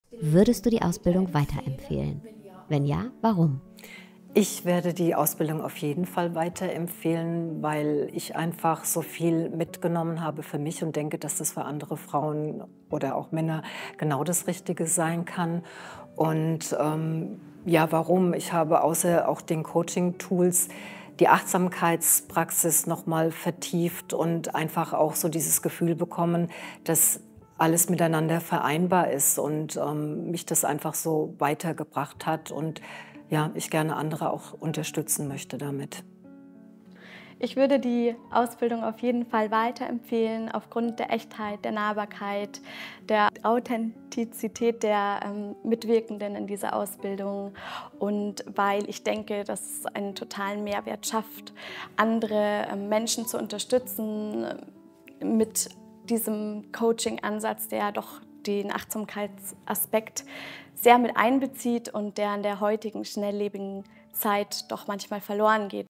Mindful Masters Academy: Nahbarkeit, Echtheit, Authentizität
Würdest du die Ausbildung weiterempfehlen? Wenn ja, warum?

Wir fragen, die Auszubildenden der vierten Ausbildungsrunde Winter 2024 antworten.

Höre dir an, was sie dazu sagen.

Weitere Infos zur Mindful Masters Academy und Coachingausbildung findest du